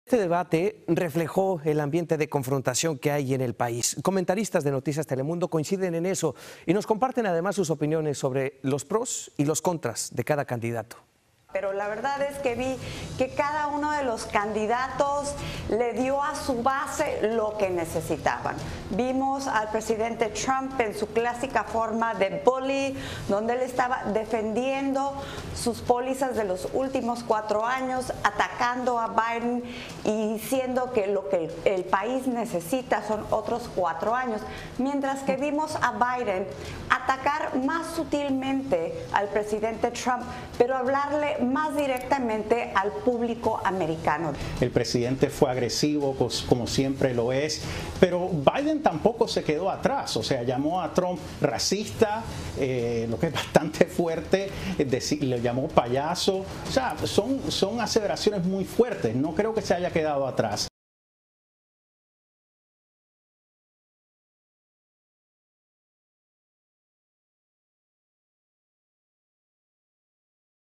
Pros y contra de las declaraciones en el primer debate | Noticias Telemundo
Video oficial de Noticias Telemundo. Comentaristas de Noticias Telemundo analizan y dan su opinión sobre los errores y aciertos de los candidatos Trump y Biden durante el primer debate presidencial.